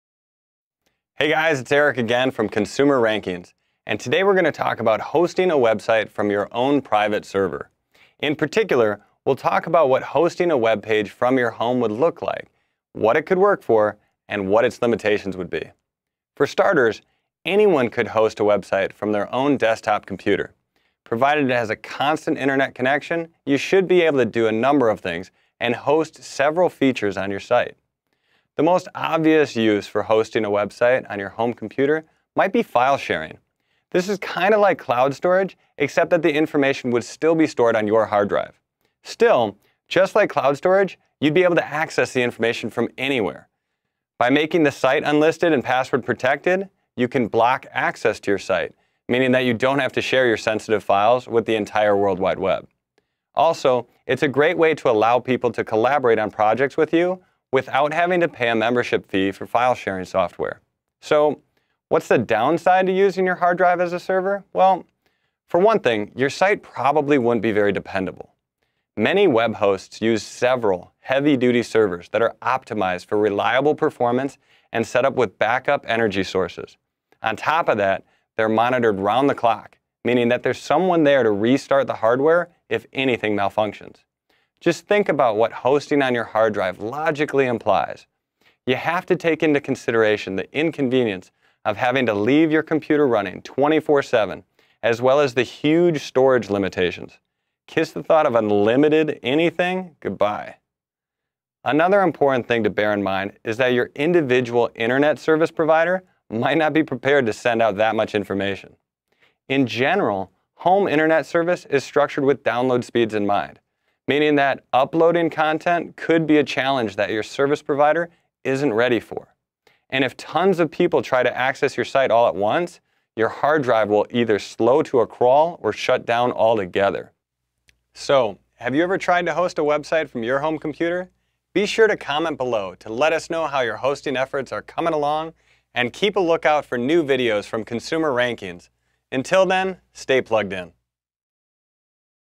Hosting a Website From Your Own Home Pros/Cons
Is your current web hosting provider not giving you enough bang for your buck? Maybe you're just the DIY type. Whichever category you fall in, hosting your website from your desktop computer might be your best bet. Many site owners are finding this solution profitable, but it does't come without its restrictions.

Consumer-Rankings wants to give you the 4-1-1 on the pros and cons of hosting your website from your home so that you know exactly what to expect. Enjoy, and feel free to drop us a line below.